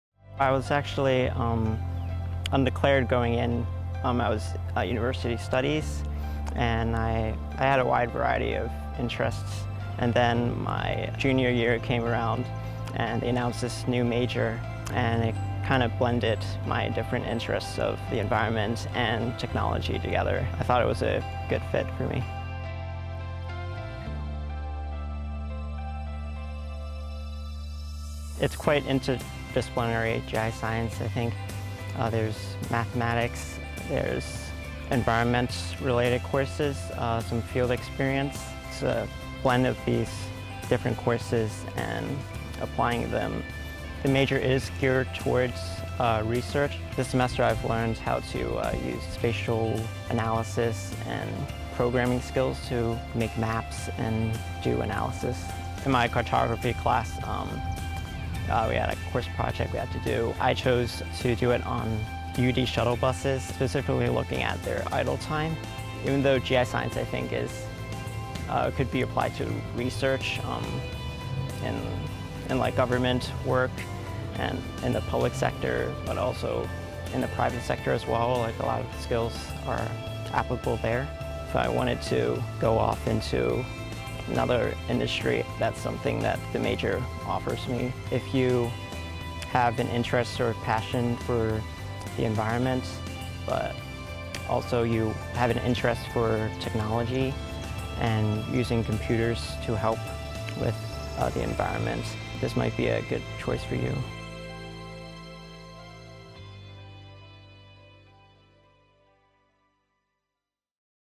Focus on Majors: GIScience and Environmental Data Analytics
Anthony Chan discusses his experience as a GI science  and environmental data analytics major at the University of Delaware's  College of Earth, Ocean and Environment, where students learn how to  analyze and harness geospatial data using computer science, electrical engineering, geography and mathematics.